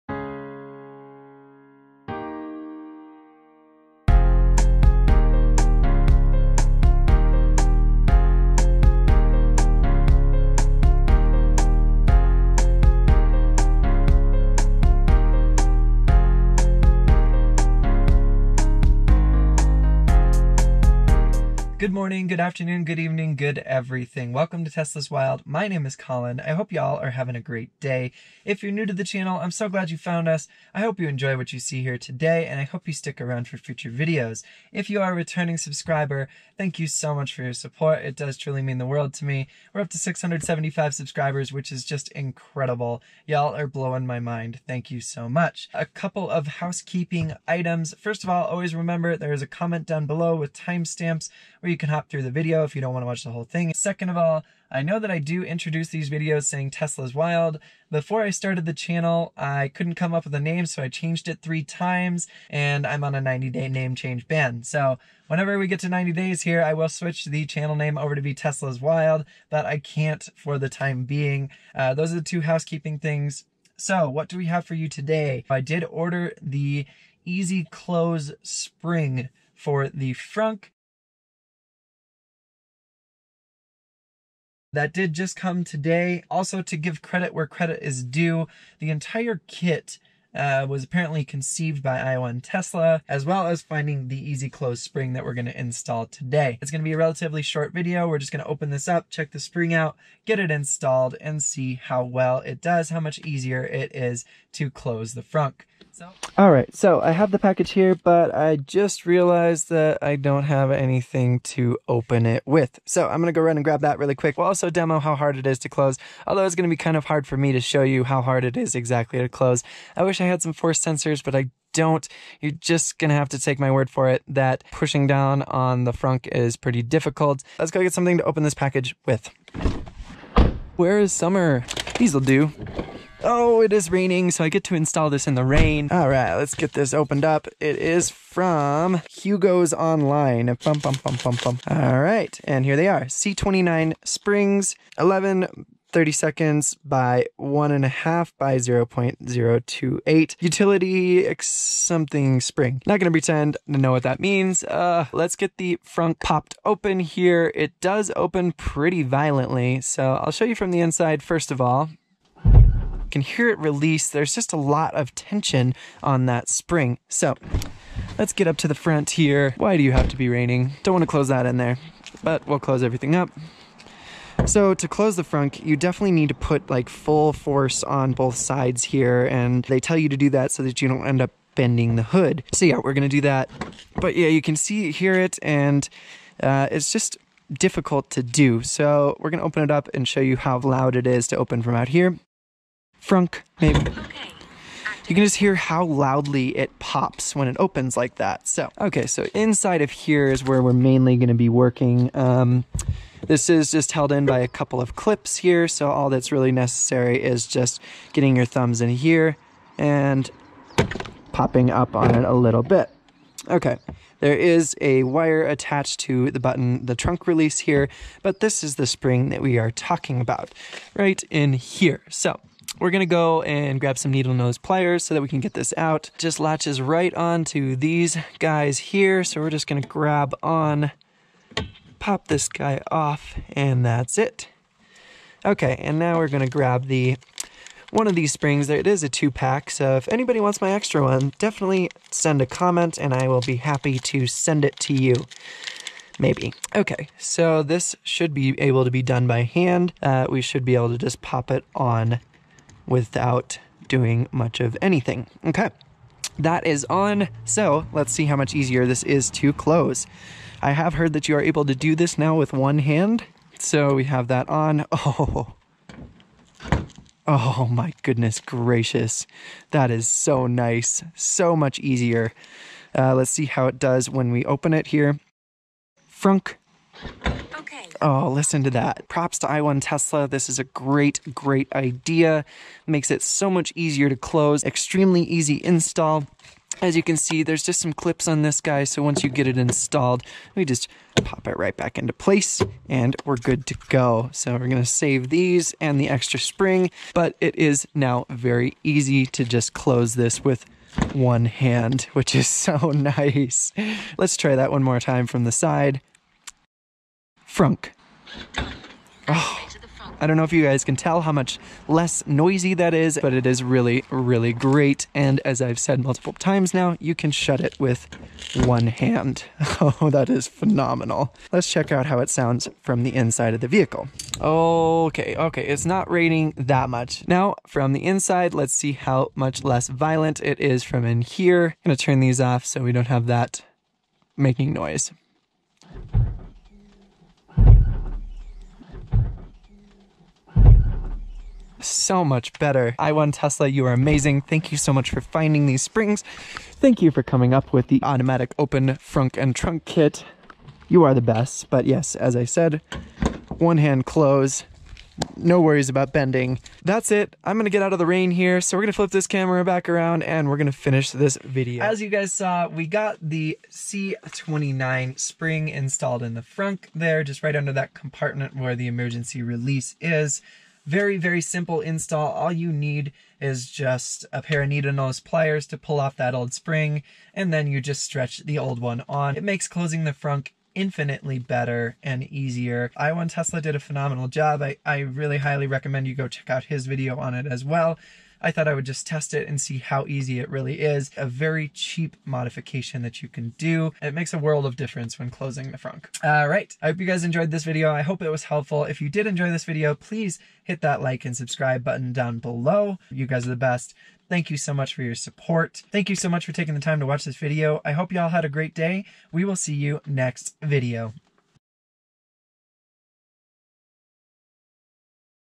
Tesla Model 3 EASY CLOSE Frunk
SEE DESCRIPTION AND COMMENTS BELOW FOR TIMESTAMPS TO HOP THROUGH VIDEO!!!

This is a vlog going over installing a new spring in the frunk that makes it much easier to close and makes it much quieter to open. It is now very easy to close the frunk with one hand, even one finger. This is well worth it for any Model 3 owner. This video was inspired by i1Tesla, he found the spring and showed the install that inspired me to give this spring a try, his video is linked below.

Springs on Amazon

Highlights:
Tesla's Wild Intro - 0:00
Opening Remarks - 0:22
Unboxing - 1:46
Pre Install Sound - 2:42
Two-Hands Close - 3:02
Pre Install Siri Demo - 3:12
POV Spring Install - 3:19
Post Install Siri Demo - 4:35
ONE HAND Close - 4:57
Post Install Siri Demo 2 - 5:04
Pre and Post Install Interior Sound Comparison - 5:35
ANOTHER ONE HAND Close - 5:48
Concluding Remarks - 6:02